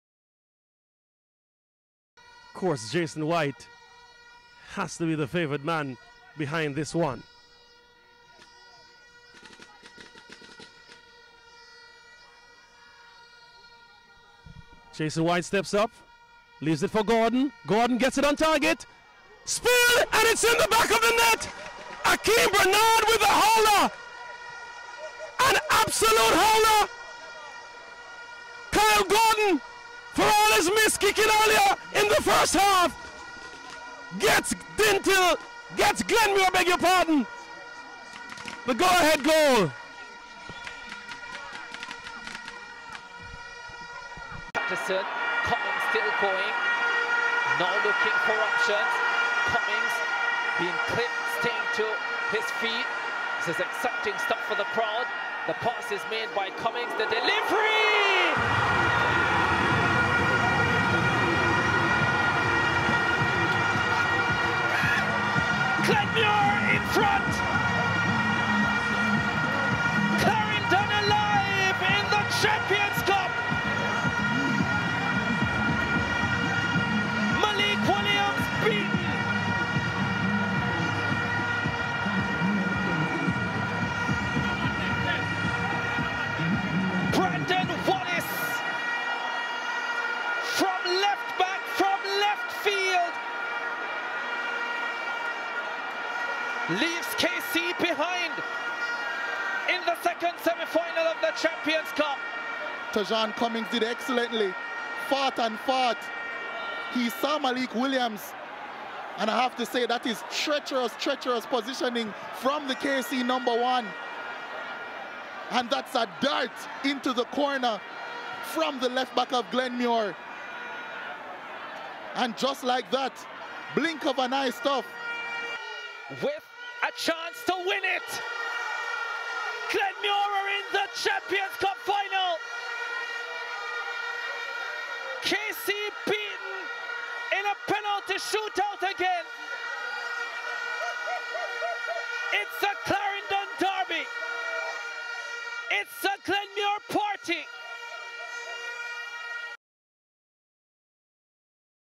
Glenmuir High’s road to the 2023 School Boy Football Champions Cup Final | SportsMax
SportsMax Tv recaps Glenmuir High's journey to the Champions Cup Final.